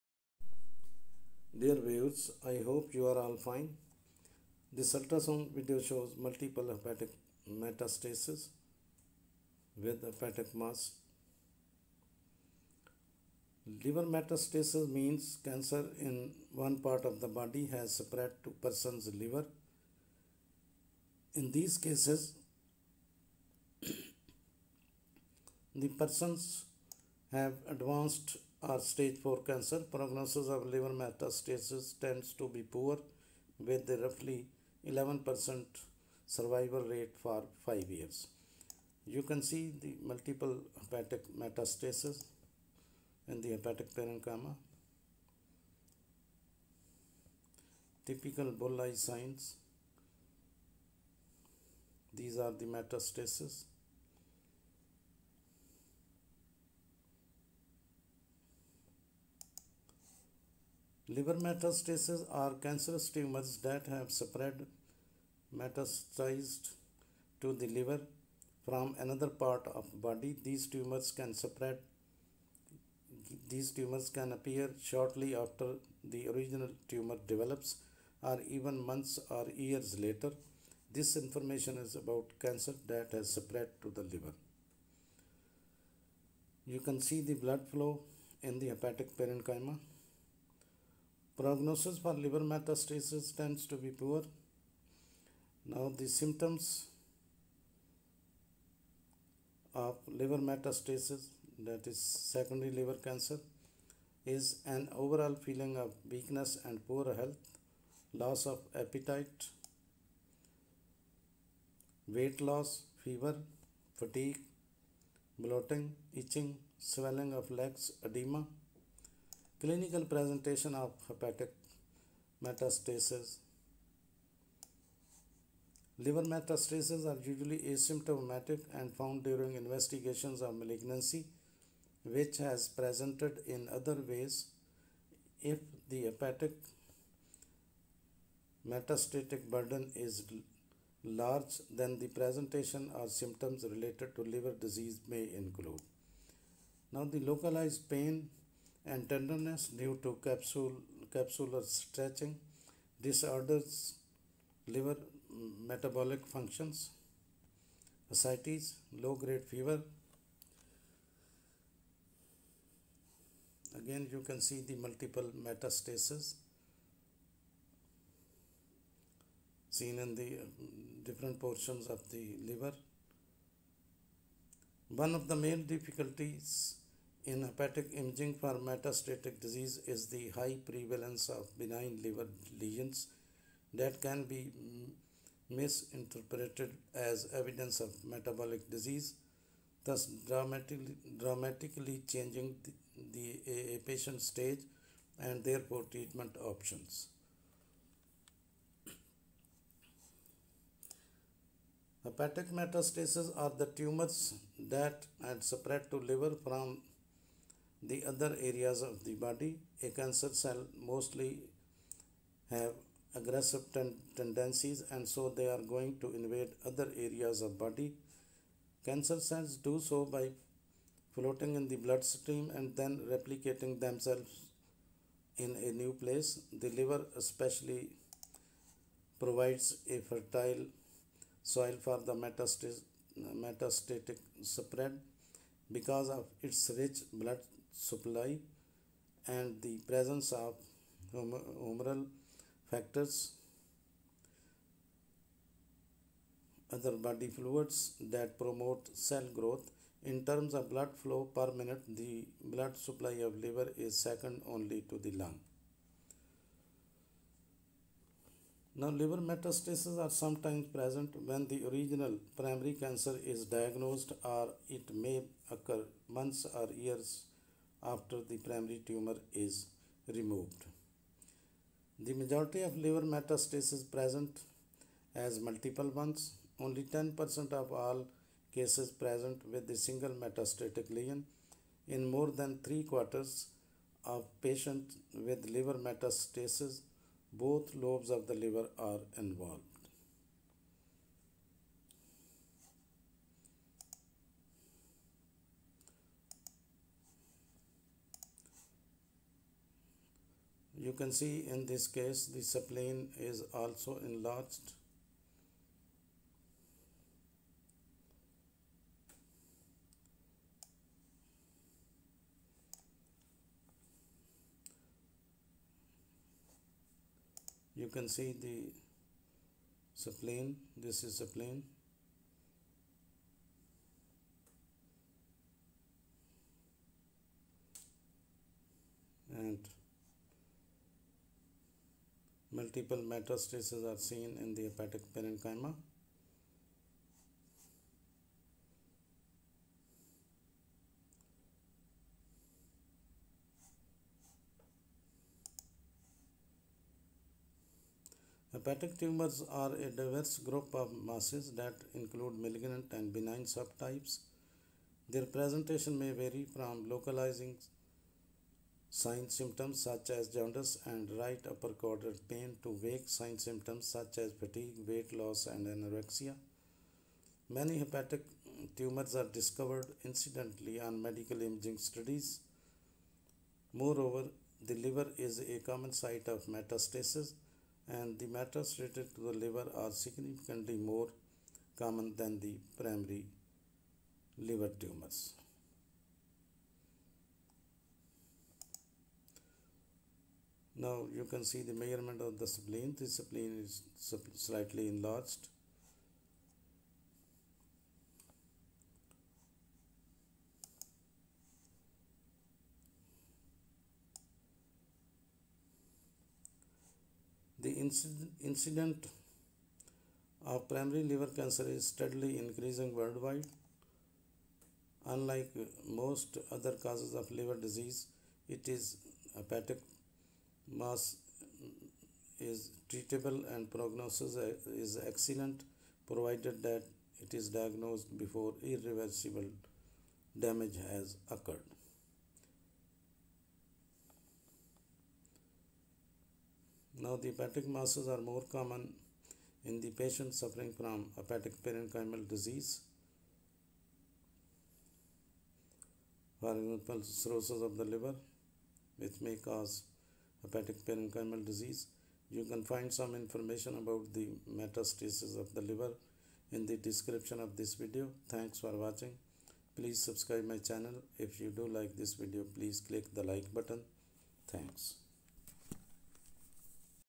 Multiple Hepatic metastases.
This video shows multiple Hepatic metastases.
Liver metastases mean that cancer in one part of the body has spread to a person's liver. In these cases, the person has advanced, or stage 4, cancer. The prognosis for liver metastases tends to be poor, with a roughly 11% survival rate for 5 years.
Liver metastases are cancerous tumors that have spread (metastasized) to the liver from another part of the body. These tumors can appear shortly after the original tumor develops, or even months or years later. This information is about cancer that has spread to the liver.
The prognosis for liver metastases tends to be poor, with a roughly 11% survival rate for 5 years. Treatments can help reduce the symptoms and shrink the tumor, but typically, there is no cure for liver metastases.
Symptoms of Liver Metastases (Secondary Liver Cancer)
• an overall feeling of weakness and poor health.
• loss of appetite.
• weight loss.
• fever.
• fatigue.
• bloating.
• itching.
• swelling of the legs (edema).
Clinical presentation of Hepatic metastasis.
Liver metastases are usually asymptomatic and found during investigations of a malignancy that has presented in other ways. If the hepatic metastatic burden is large then the presentation or symptoms related to the liver disease may include:
Localized pain and tenderness due to capsular stretching
Disordered liver metabolic function
Ascites
Low-grade fever
One of the main difficulties in hepatic imaging for metastatic disease is the high prevalence of benign liver lesions that can be misinterpreted as evidence of metastatic disease, thus dramatically changing a patient's stage, and therefore treatment options.
Hepatic metastases are the tumors that had spread to the liver from other areas of the body. As cancer cells mostly have aggressive tendencies and so they are going to invade other areas of the body. Cancer cells do so by floating in the bloodstream and then replicating themselves in a new place. The liver, especially, provides fertile soil for metastatic spread because of its rich blood supply and the presence of humoral factors (other bodily fluids) that promote cell growth. In terms of blood flow per minute, the blood supply of the liver is second only to the lung.
Liver metastases are sometimes present when the original (primary) cancer is diagnosed, or it may occur months or years after the primary tumor is removed.
The majority of liver metastases present as multiple ones. Only 10% of all cases present with a single metastatic lesion. In more than three-quarters (3/4) of patients with liver metastases, both lobes of the liver are involved.